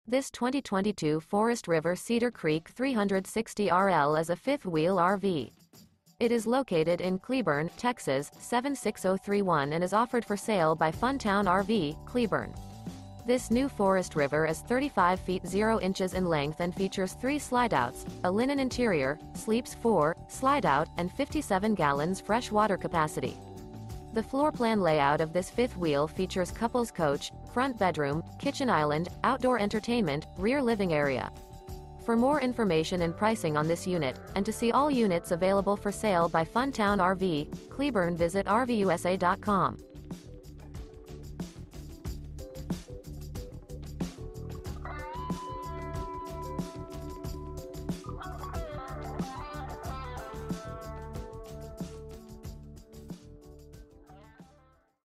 Breathtaking 2022 Forest River Cedar Creek Fifth Wheel RV For Sale in Cleburne, TX | RVUSA.com
This 2022 Forest River Cedar Creek 360RL is a Fifth Wheel RV. It is located in Cleburne, TX 76031 and is offered for sale by Fun Town RV - Cleburne. This New Forest River Is 35' 0" in length and features 3 slideouts, a LINEN interior, sleeps 4, Slideout, and 57 gal fresh water capacity. The floorplan layout of this Fifth Wheel features Couples Coach, Front Bedroom, Kitchen Island, Outdoor Entertainment, Rear Living Area.